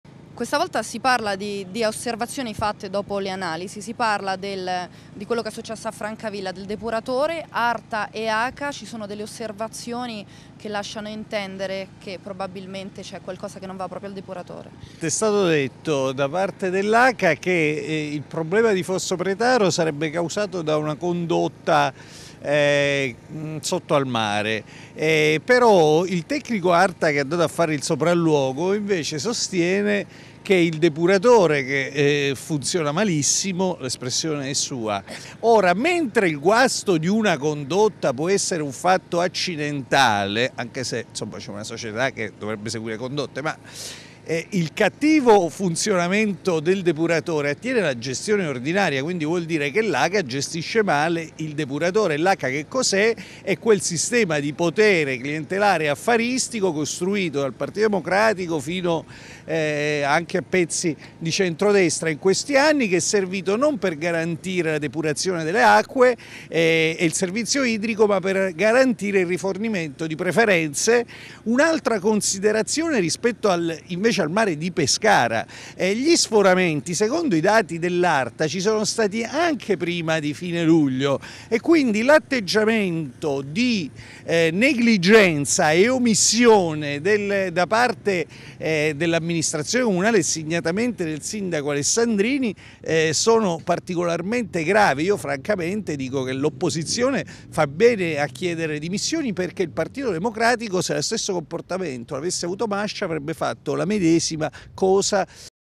Acerbo Arta denuncia il cattivo funzionamento del depuratore a Francavilla
L' esponente di rifondazione comunista, Maurizio Acerbo, rende pubblica una denuncia fatta dall arta sul cattivo funzionamento del depuratore di Francavilla. Secondo Acerbo tutto è riconducibile alla cattiva gestione dell azienda comprensoriale acquedottistica, l Aca.